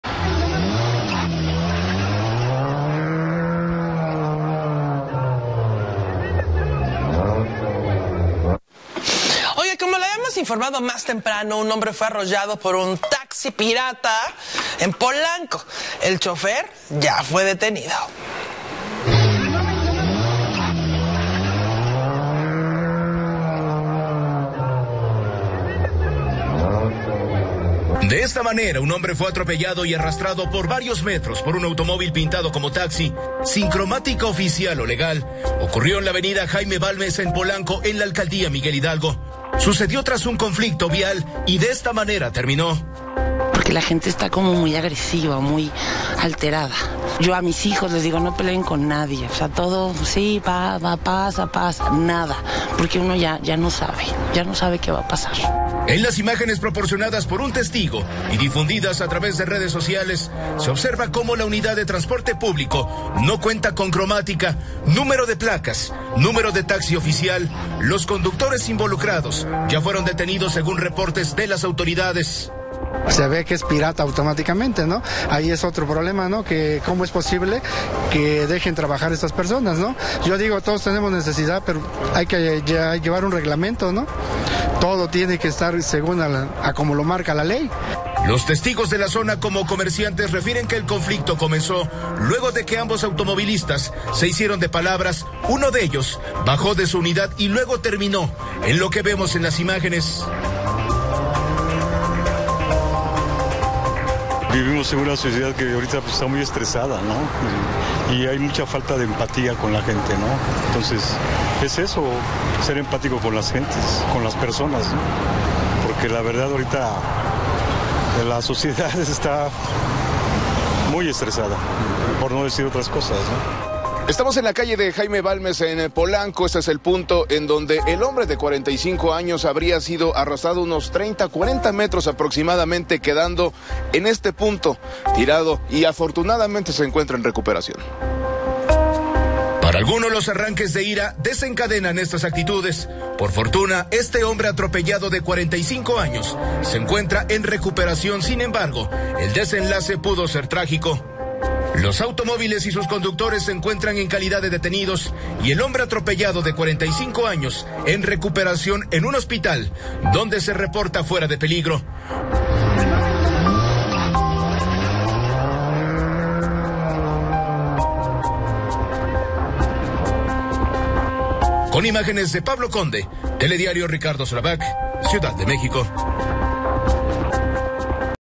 Taxista atropella a hombre en CdMx tras discusión en Polanco
Un hombre de 45 años de edad, fue arrollado por un vehículo tipo taxi en la colonia Polanco Primera Sección, de la alcaldía Miguel Hidalgo, la tarde de ayer, informó la Secretaría de Seguridad Ciudadana de la Ciudad de México.